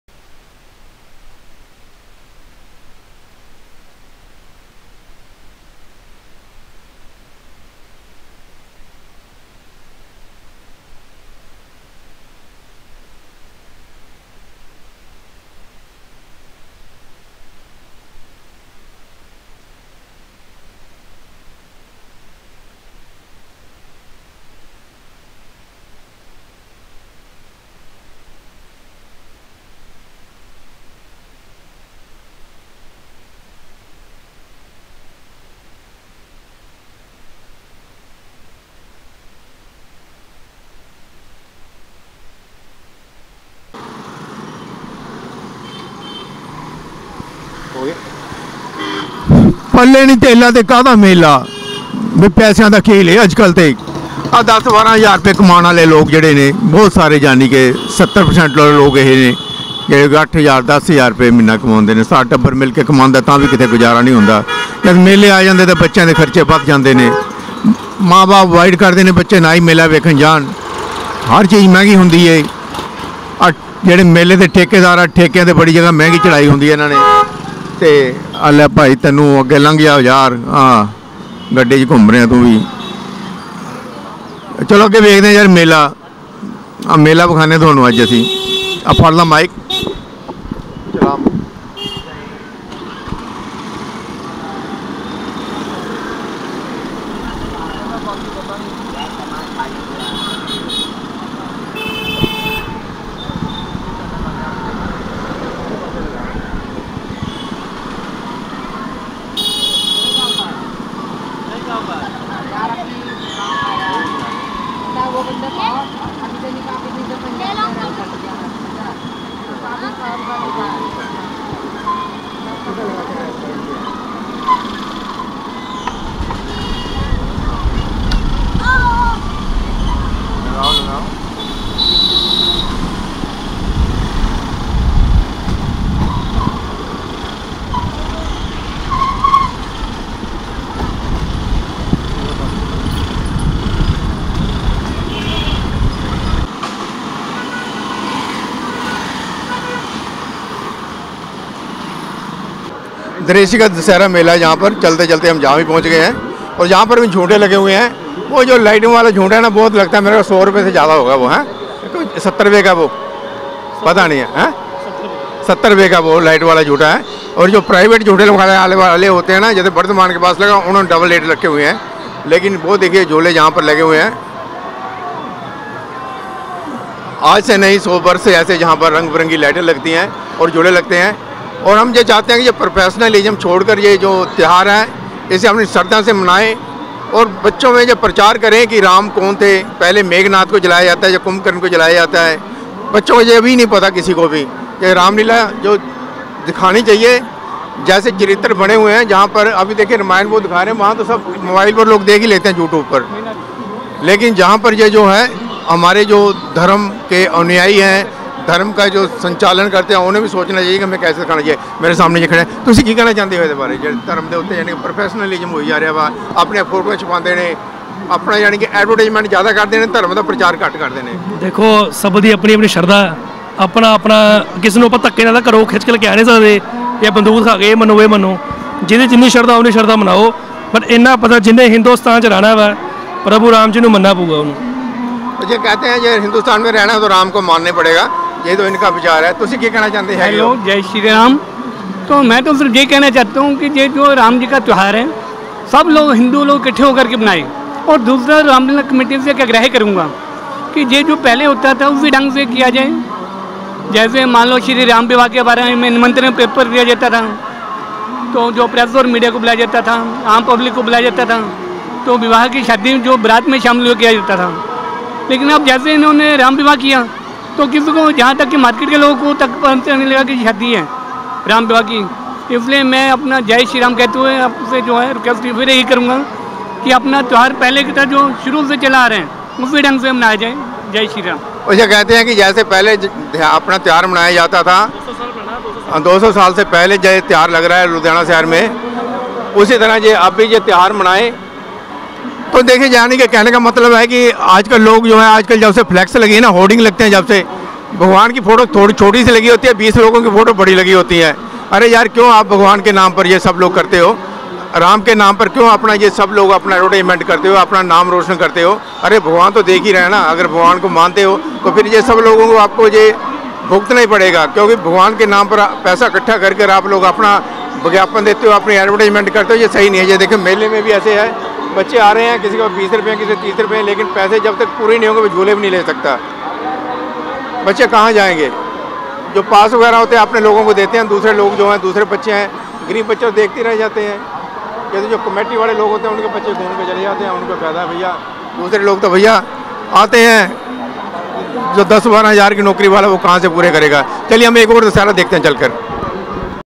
हिन्दू लोग मेले में धर्म प्रचार नहीं, करते है अपना प्रचार | Hindu Festival | Ludhiana City Punjab
In Ludhiana, the Dussehra festival is celebrated in a number of places, but this year civil lines community also celebrates Ramleela in Glada ground at the old district court ground. The active members of the committee performed Ganpati Pooja with all Ram devotees with Hindu Vedic tradition.